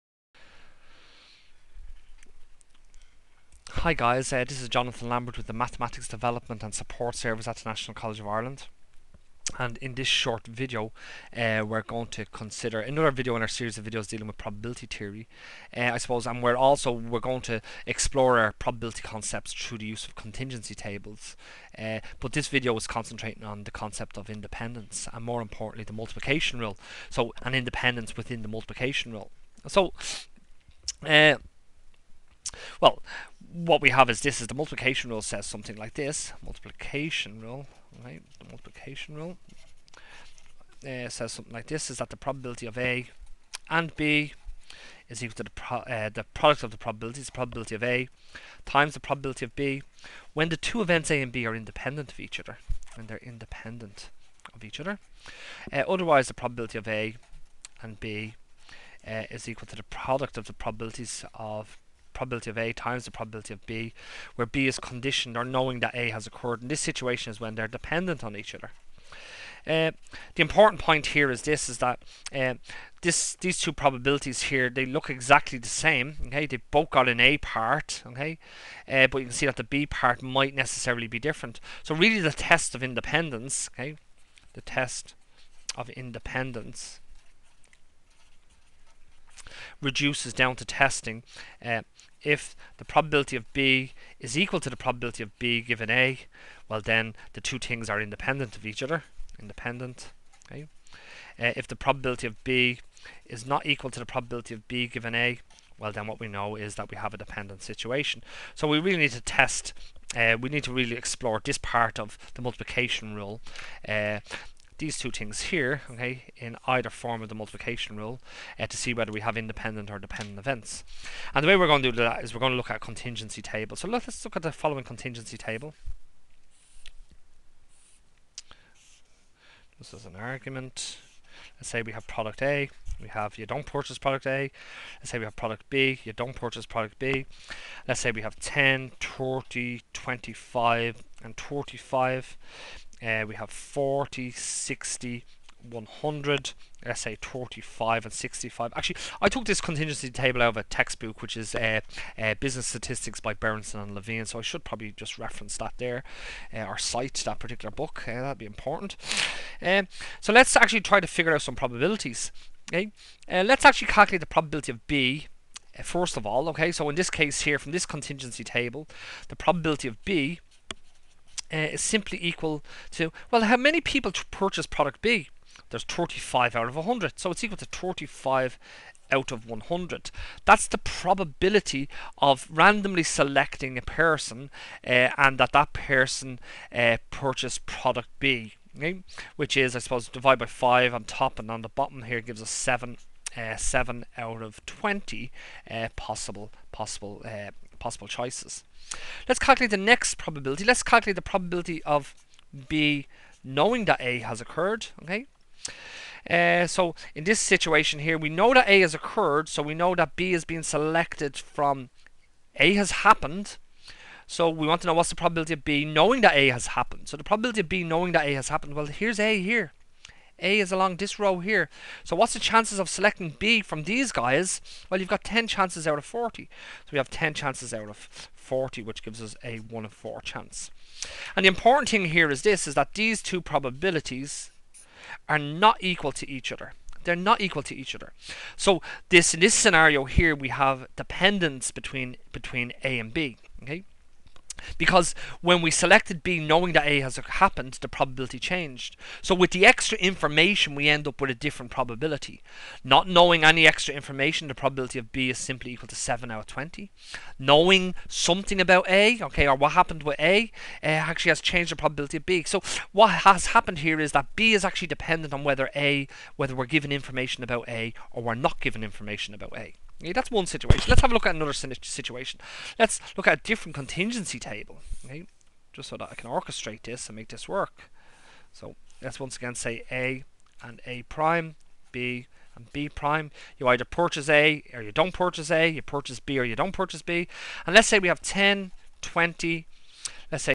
Probability Theory: Contingency Tables - Independence - Part 1
In this short video we explore the concept of statistical independence and dependence. In particular, we present the concept through the use of contingency tables and explore the consequences for the multiplication rule.